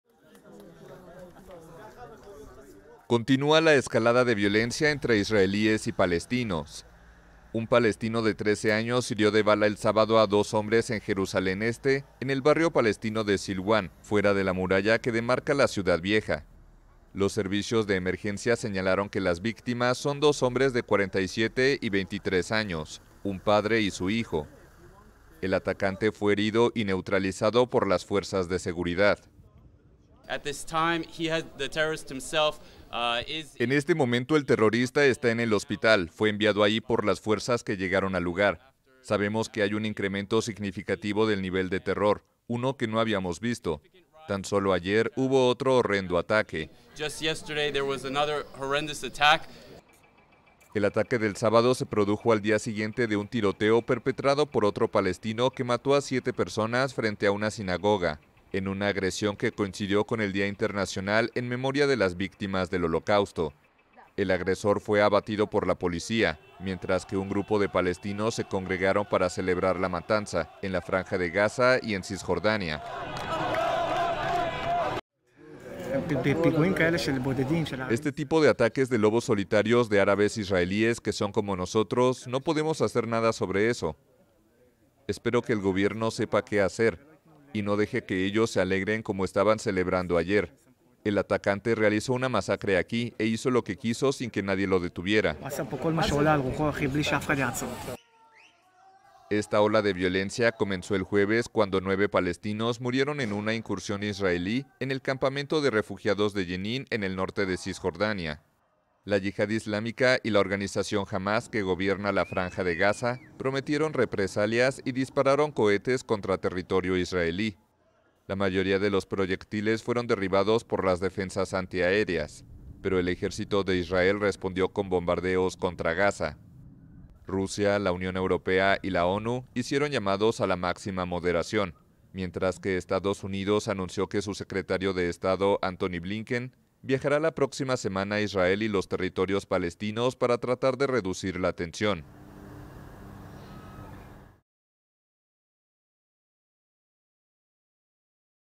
Nuevo ataque en Jerusalén Este eleva la tensión entre israelíes y palestinos | AFP
Un palestino de 13 años hirió de bala a dos hombres en Jerusalén Este el sábado, al día siguiente de que otro palestino matara a siete personas frente a una sinagoga, en uno de los peores ataques en Israel en los últimos años.